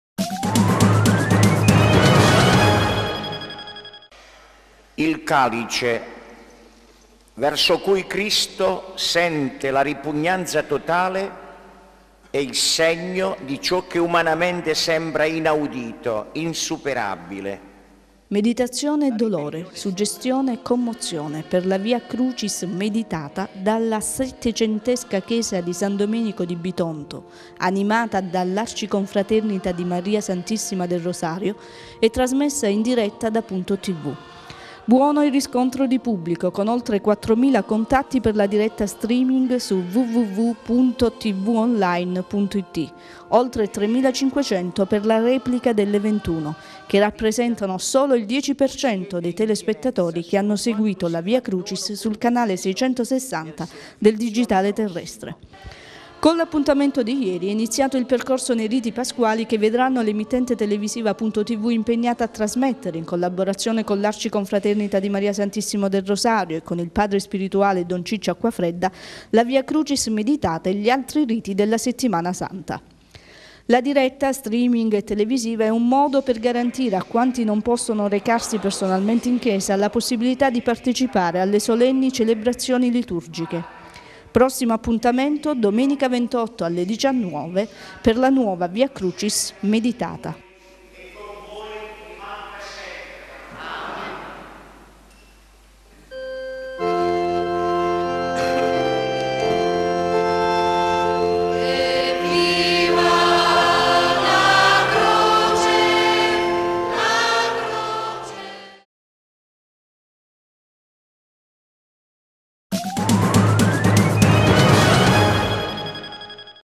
Via Crucis Meditata, successo per la diretta
Meditazione e dolore, suggestione e commozione per la Via Crucis Meditata dalla settecentesca chiesa di San Domenico di Bitonto, animata dall’Arciconfraternita di Maria SS. Del Rosario e trasmessa in diretta da PuntoTV. Buono il riscontro di pubblico, con oltre 4000 contatti per la diretta streaming su oltre 3500 per la replica delle 21, che rappresentano il 10% dei telespettatori che hanno seguito la Via Crucis sul canale 660 del digitale terrestre.
Con l’appuntamento di ieri è iniziato il percorso nei riti Pasquali che vedranno l’emittente televisiva PuntoTV impegnata a trasmettere, in collaborazione con l'Arciconfraternita di Maria SS. del Rosario e con il Padre Sprituale, Don Ciccio Acquafredda,  la Via Crucis Meditata e gli altri riti della Settimana Santa.
La diretta, streaming e televisiva, è un modo per garantire, a quanti non possono recarsi personalmente in chiesa, la possibilità di partecipare alle solenni celebrazioni liturgiche. Prossimo appuntamento domenica 28 alle 19 per la seconda Via Crucis Meditata.